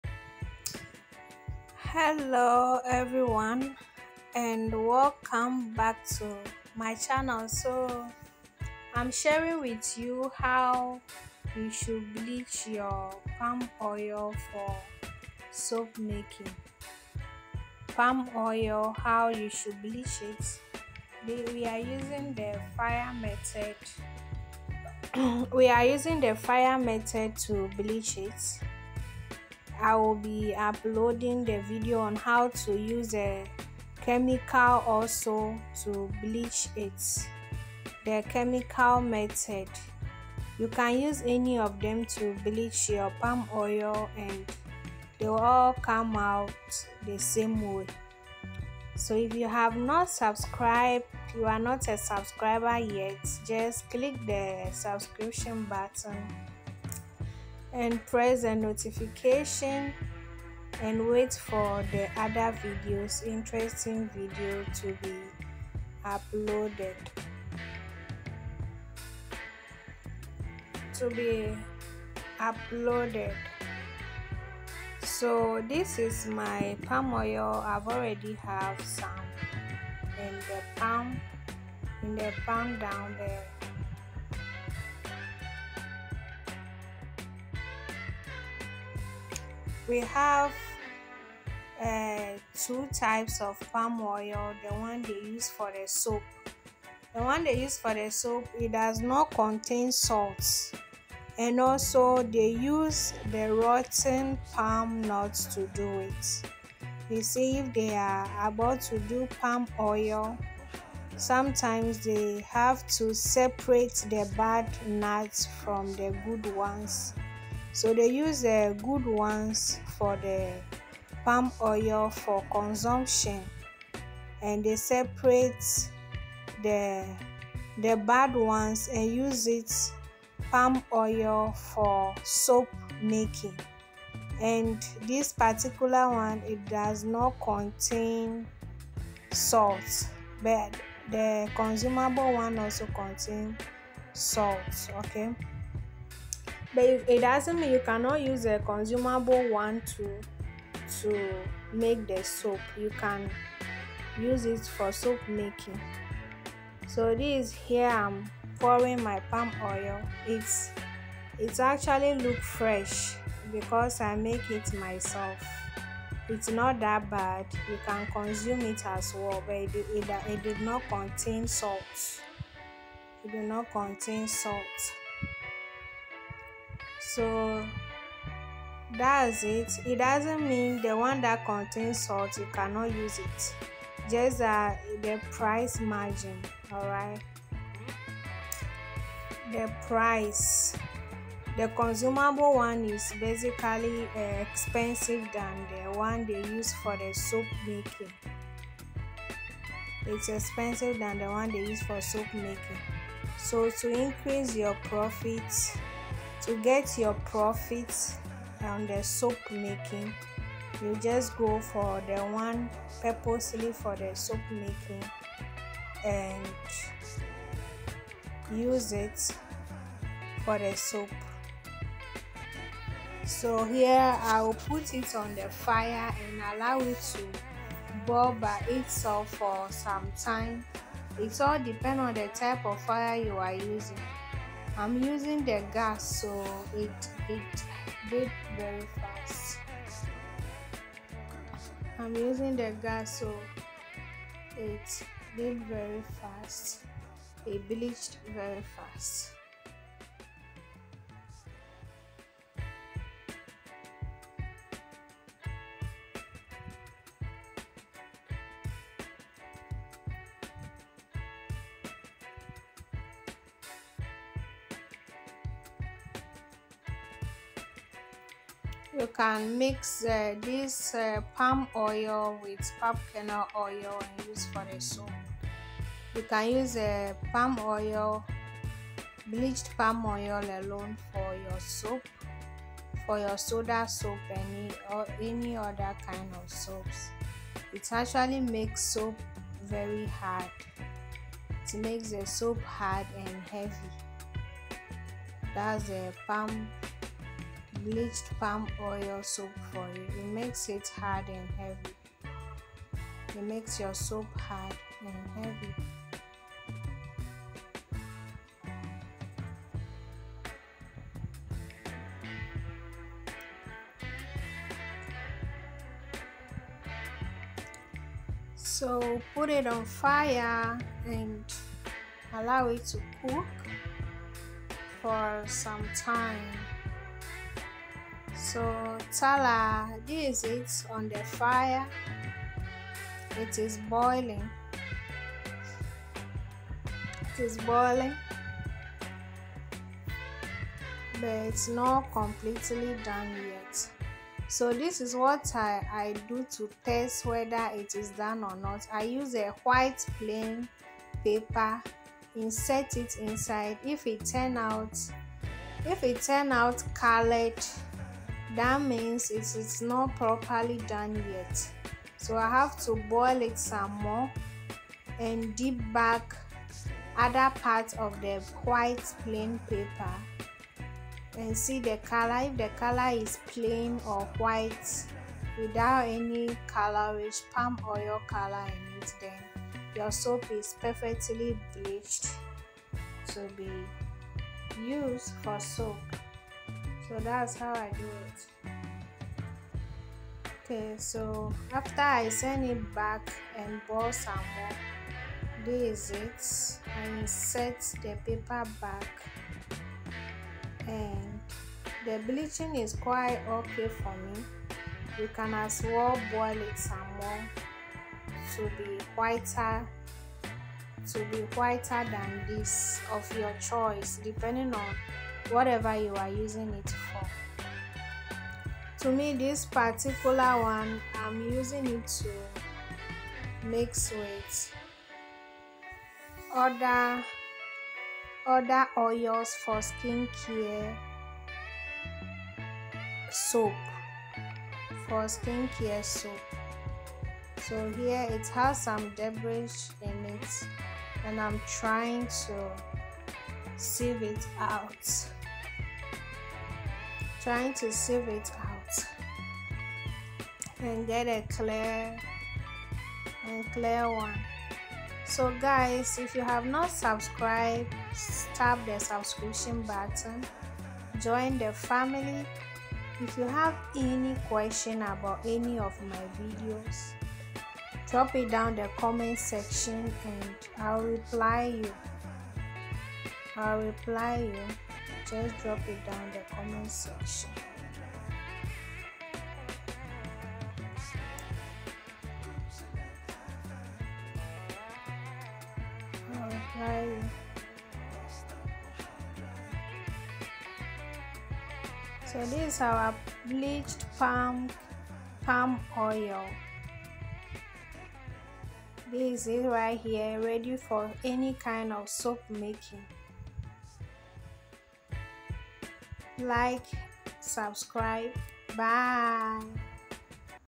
HOW TO BLEACH PALM OIL FOR SOAP MAKING.
welcome once again to this channel, today we are bringing you the easiest and fastest way to bleach palm oil for soap making at home. so please watch the video to see how i did it. thank you.